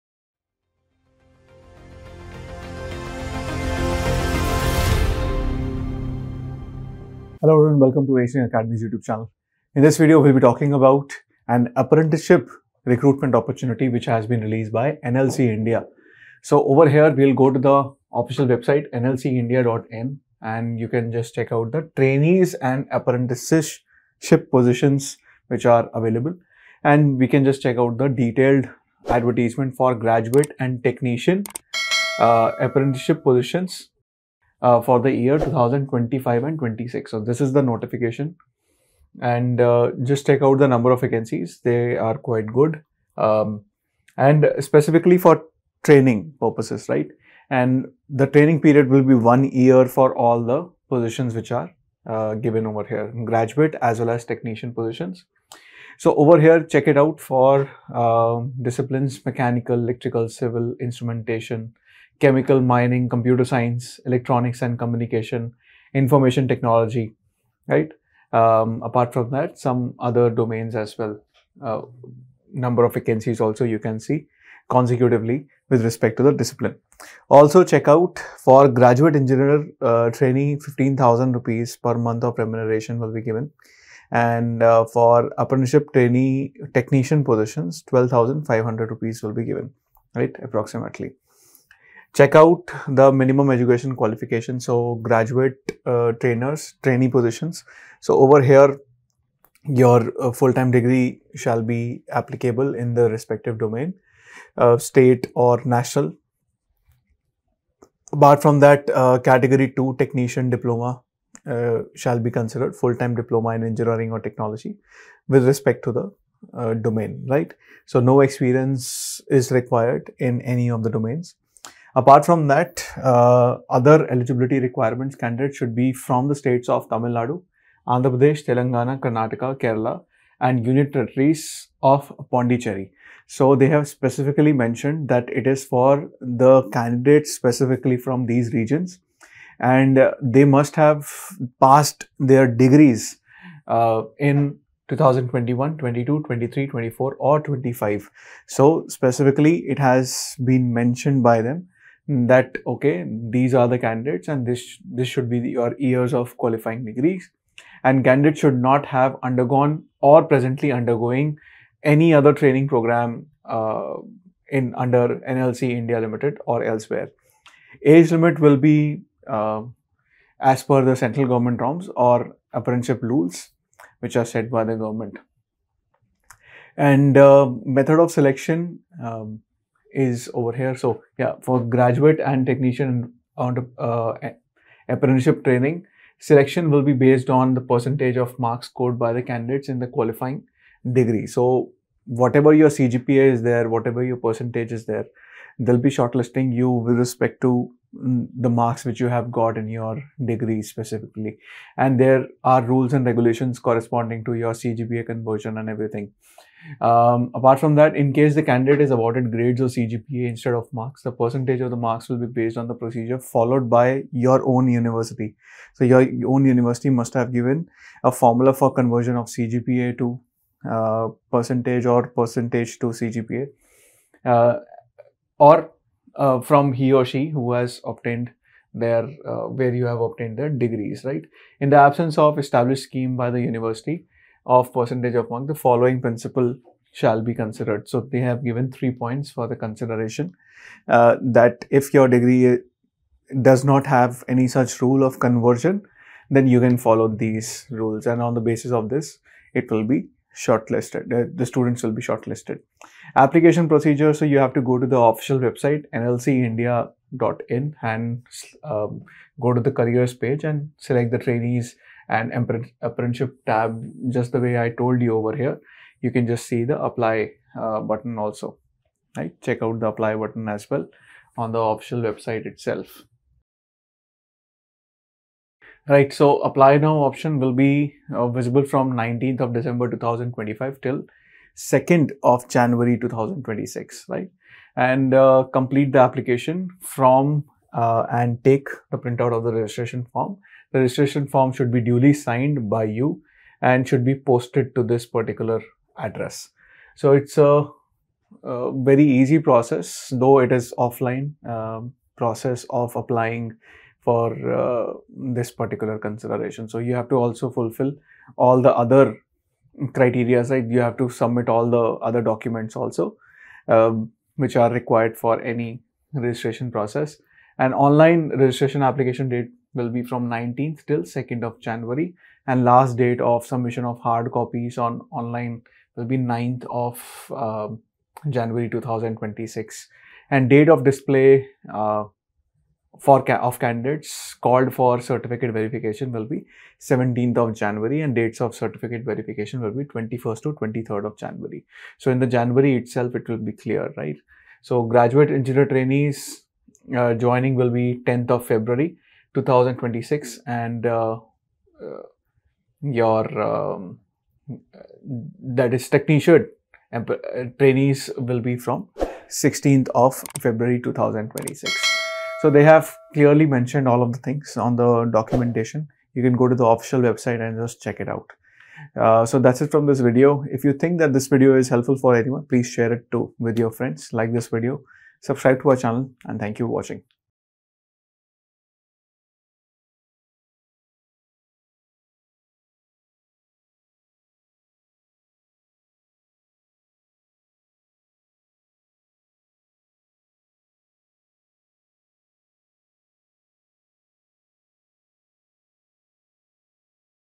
NLC India Limited Apprentices Recruitment 2025 | Vacancies: 500+ | ACE Online
NLC India Limited Apprentices Recruitment 2025 is now open! A Navratna Public Sector Enterprise invites applications for 500+ vacancies under GAT-TAT. Eligible candidates with B.Tech in ME, EE, CE, CS, or EC can apply. Don’t miss this golden opportunity to start your career with a prestigious organization. Apply online before the last date: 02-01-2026. Check eligibility, pay scale, and application details now!